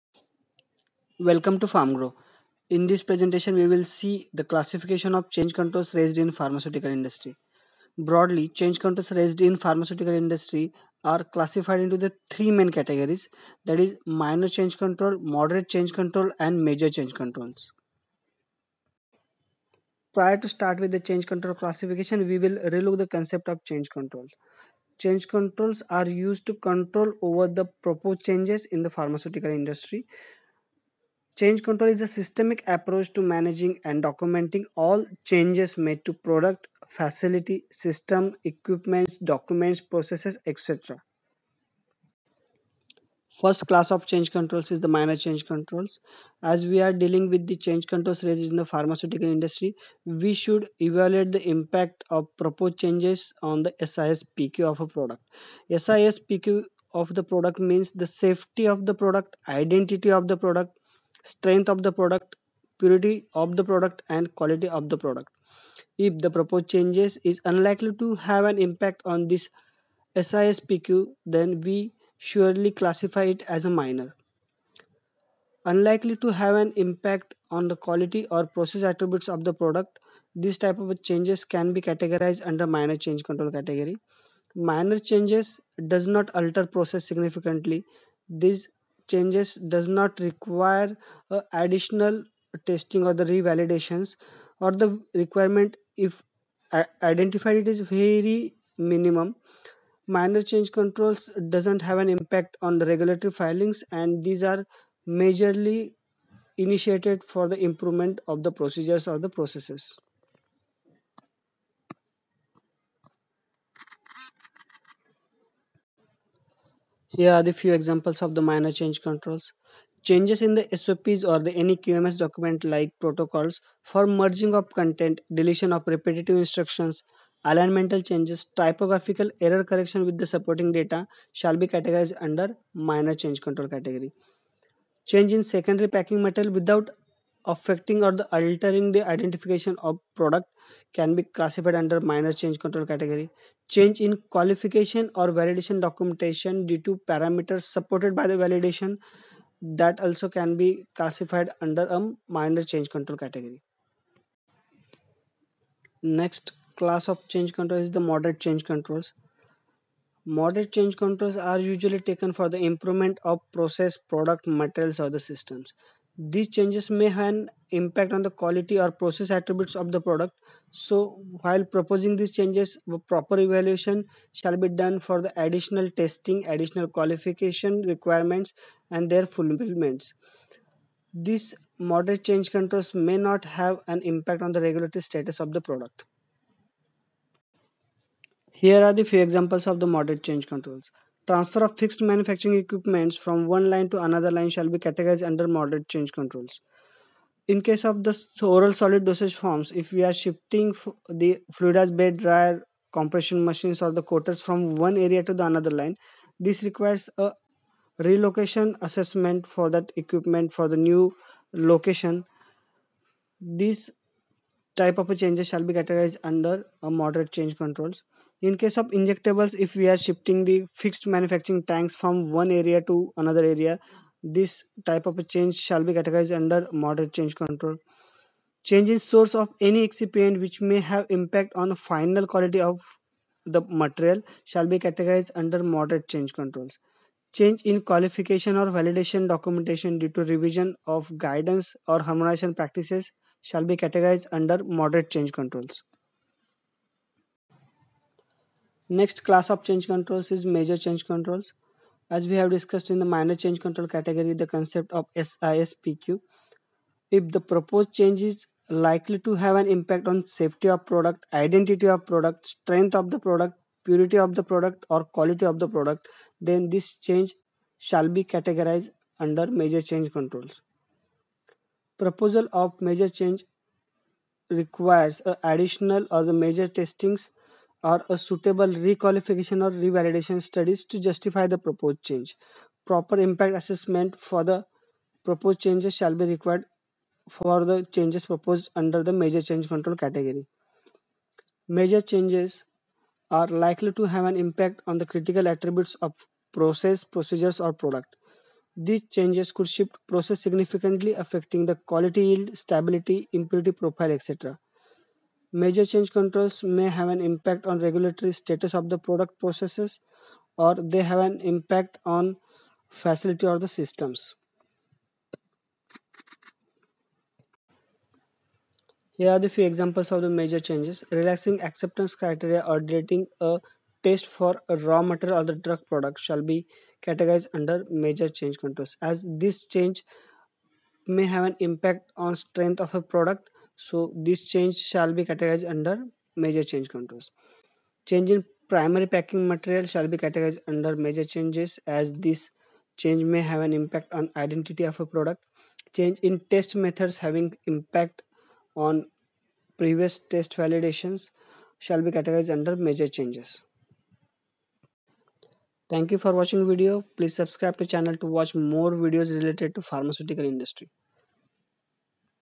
Change control classification in pharmaceutical industry. Minor, moderate and Major change controls.
In this video we will see how to evaluate and classify change control raised in pharmaceutical industry. Basic categories along with the examples has been covered in this video.
1. Minor change
2. Moderate change
3. Major change

Time stamps:

00:00 Introduction
00:19 What is change control
00:43 Minor change control
02:34 Moderate change controls
04:07 Major change controls